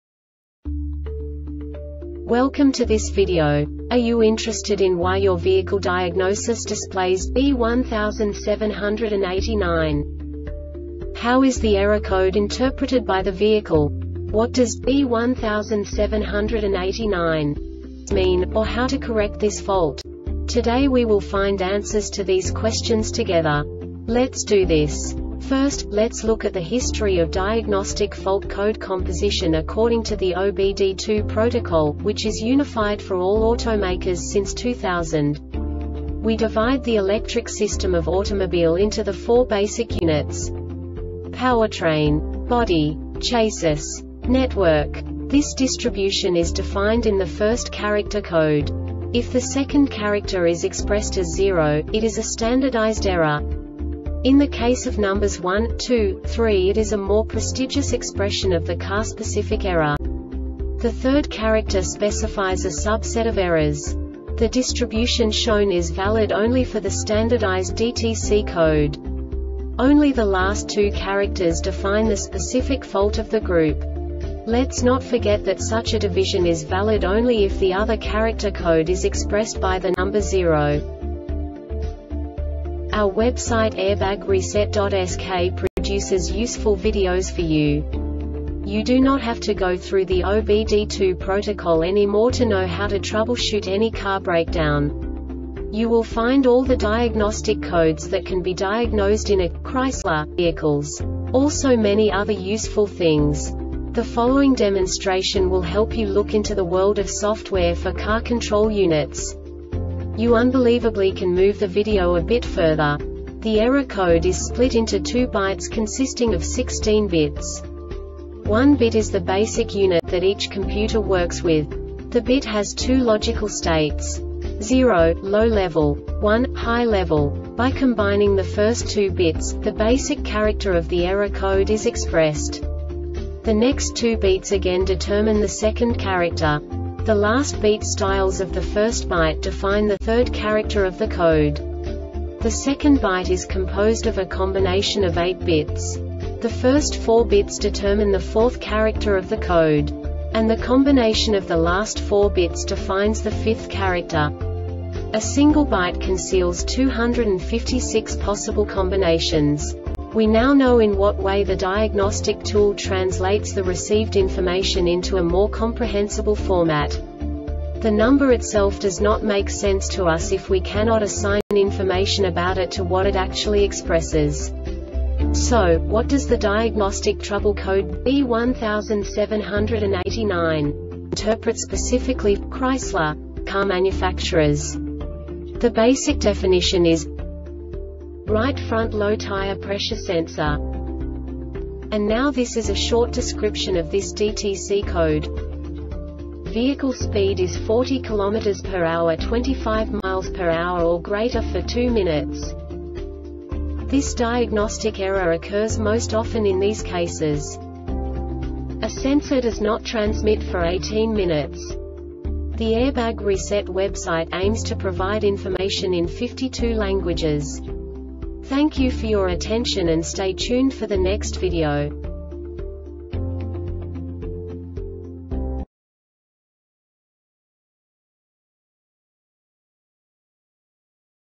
DTC Chrysler B1789 Short Explanation
Contents:
0:21 Basic DTC analysis according to OBD2 protocol standard.
1:48 Insight into programming
2:58 A diagnostically understandable description of the Diagnostic Trouble Code
3:05 Basic error definition

Make:
Chrysler
Code:
B1789
Definition:
Right Front Low Tire Pressure Sensor
Description: